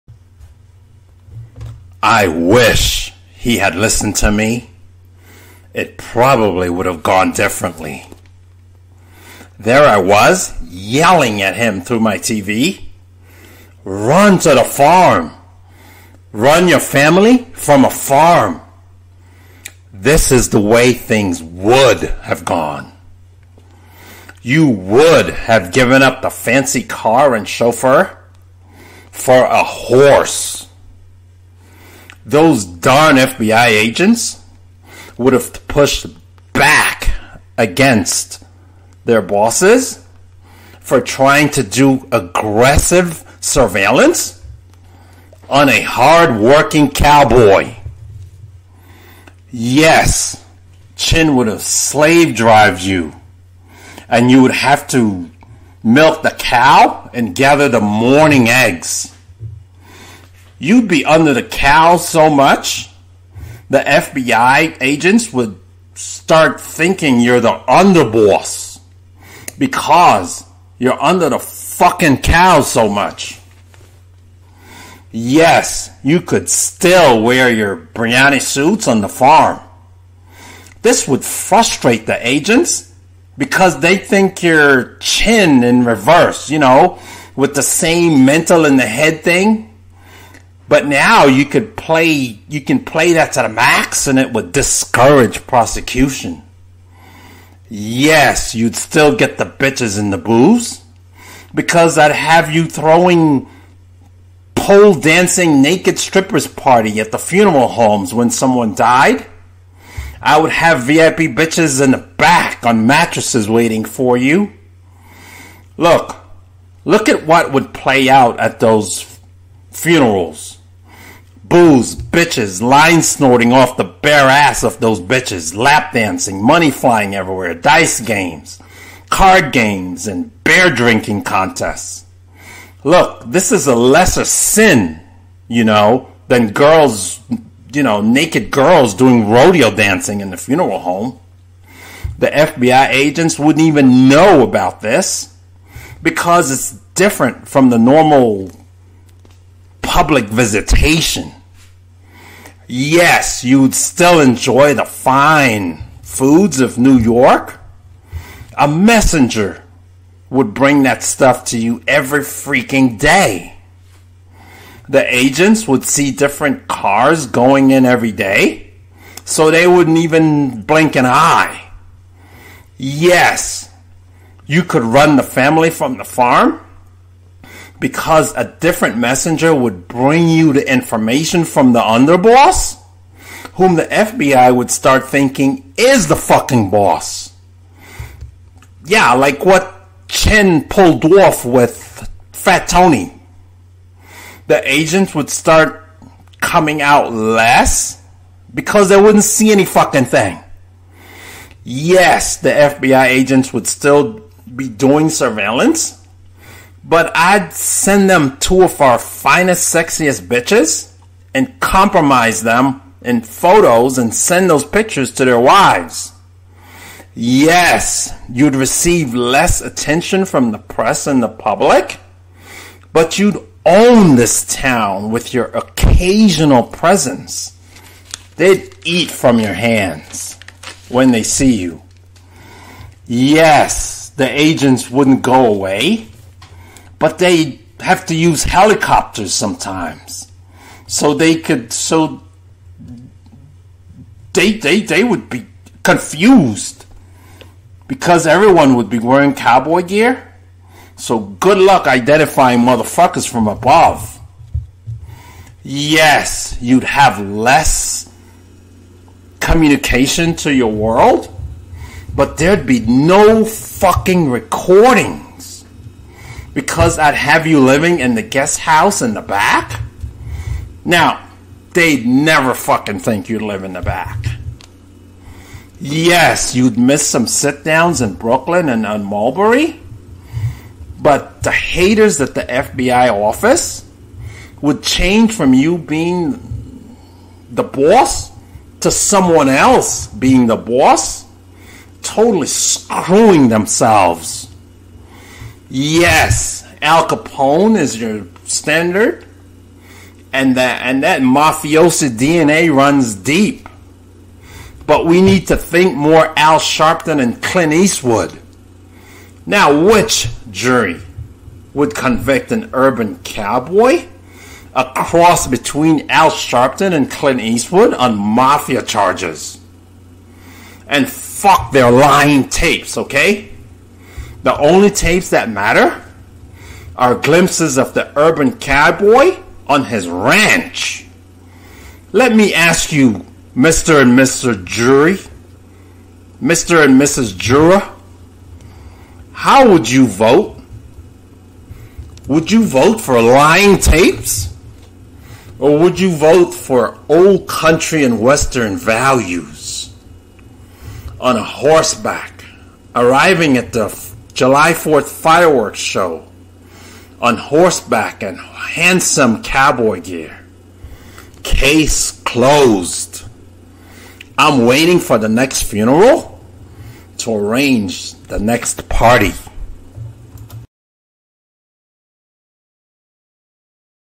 Mafia New York Cowboy
John Gotti could have ruled the Gambino family from a farm.  A jury would less likely convict a cowboy on mafia charges.